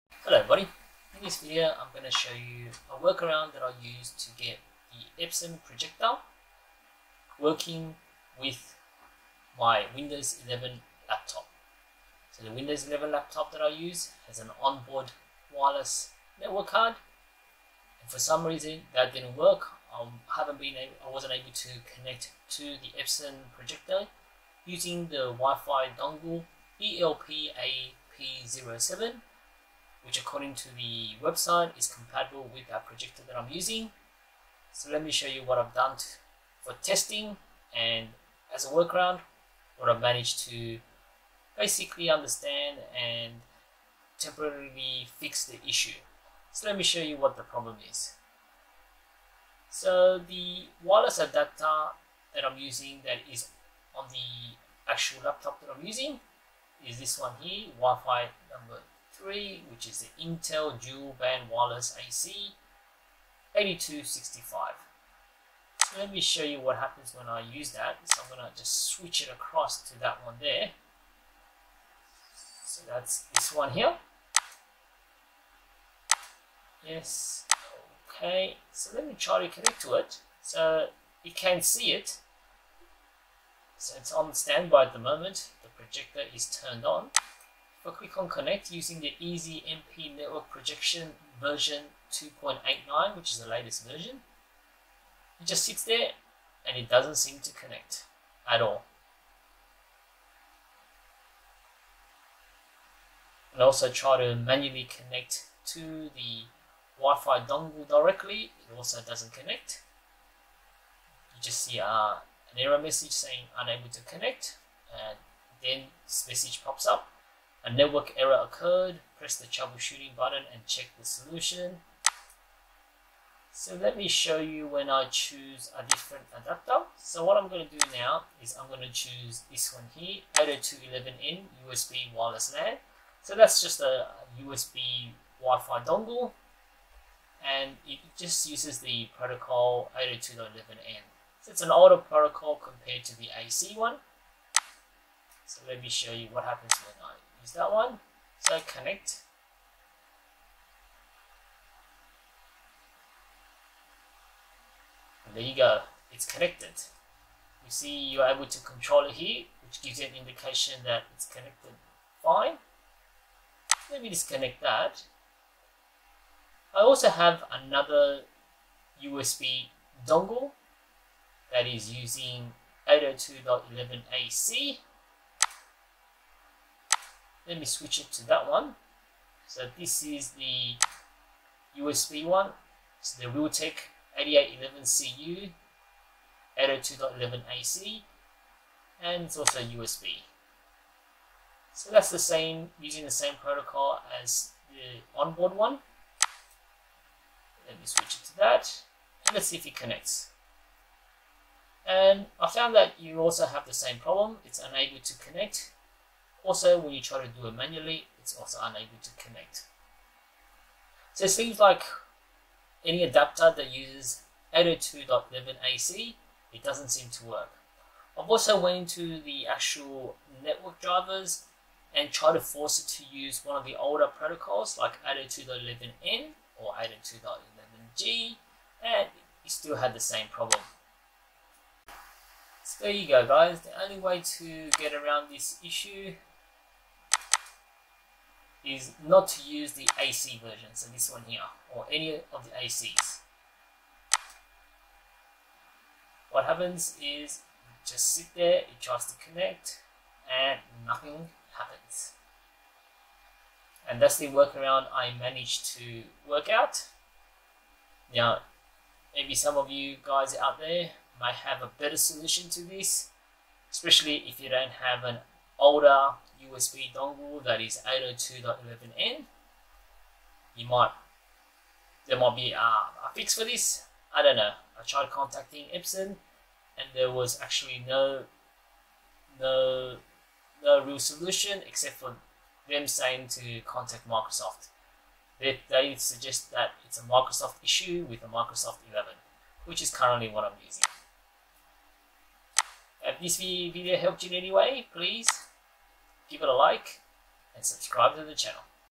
HOW TO FIX ISSUE WITH EPSON PROJECTOR AND EASYMP CONNECTIVITY ON WINDOWS 11
In this video, I will show you what I did to fix an issue with connecting to the Epson Projector using EasyMP with Windows 11.

Sorry about the audio, for some reason I was using the wrong microphone during the recording.

The Epson USB wireless dongle used is ELPAP07 (also tested with ELPAP10 with the same issue).

The error message from EasyMP is:

A network error occurred. Press the Troubleshooting button and check the solution (KF0302B).

The issue: I tried connecting from my Windows 11 laptop using the inbuild wireless card to the Epson Projector USB dongle with the Epson Easy MP version 2.89. It failed and couldn't connect.

When I tested on a different device with Windows 7 it worked so I thought maybe the Windows OS version cause the issue.

Eventually, I found that it was a combination of Windows 10 and that the type of wireless adaptor (mainly using 802.11AC) didn't work.

The solution was to use another USB external adaptor with 802.11N.

You can get generic 802.11N adaptors (avoid the newer model with 802.11AC).

OUTLINE:

00:00 - Intro
00:28 - Epson wireless dongle ELPAP07
00:58 - Epson EasyMP software
01:03 - Internal Dual Band Intel Wireless-AC 8203
01:07 - Switch LAN
01:36 - Epson EasyMP software v2.89
01:53 - A network error occurred message
02:10 - Switch to 802.11n adapter
02:37 - Working and connected
03:01 - Switch to 802.11AC USB adapter
03:45 - Failed to connect
04:37 - Epson response